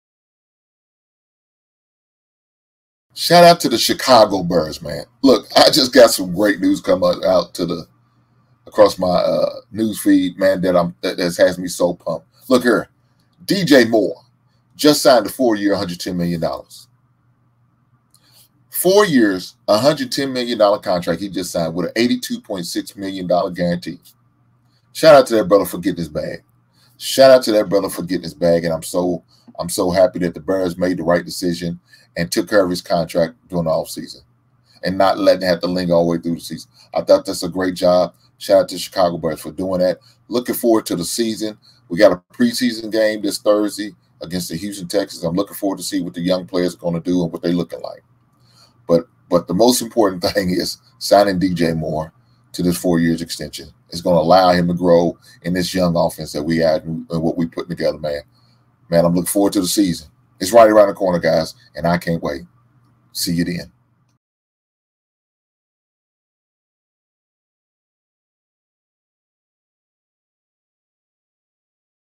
The Bears signed WR DJ Moore to a four year extension
The Chicago Bears are signing wide receiver DJ Moore to a four-year, $110-million extension that includes $82.6 million guaranteed.